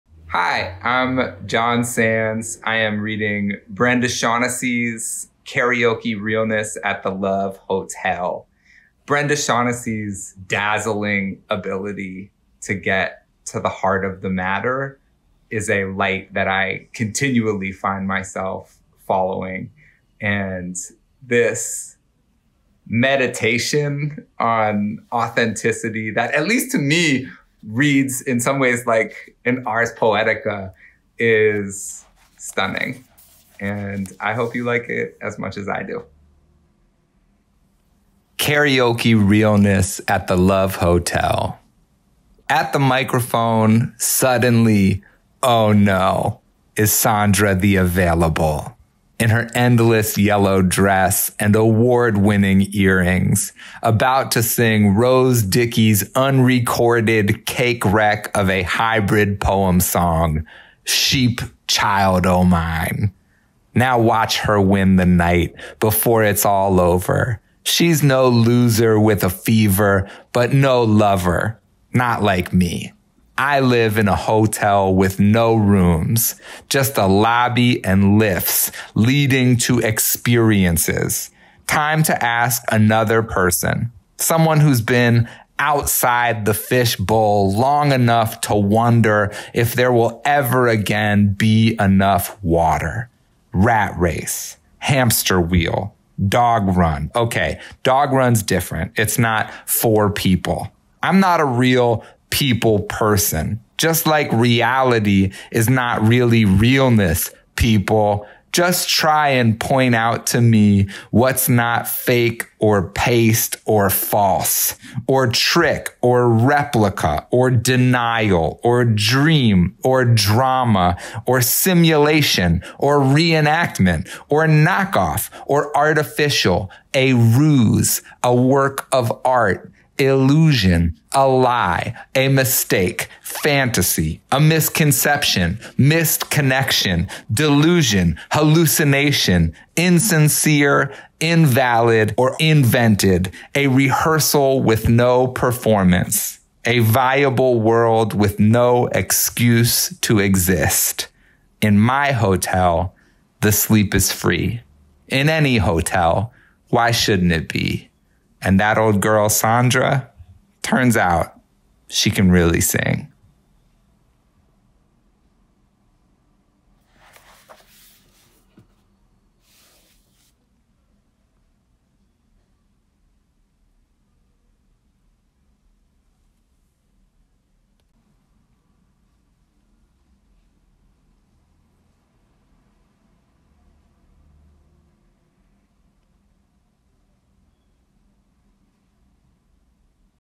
Jon Sands reads "Karaoke Realness at the Love Hotel" by Brenda Shaughnessy
Jon Sands (he/him/his) reads Brenda Shaughnessy's poem, “Karaoke Realness at the Love Hotel.”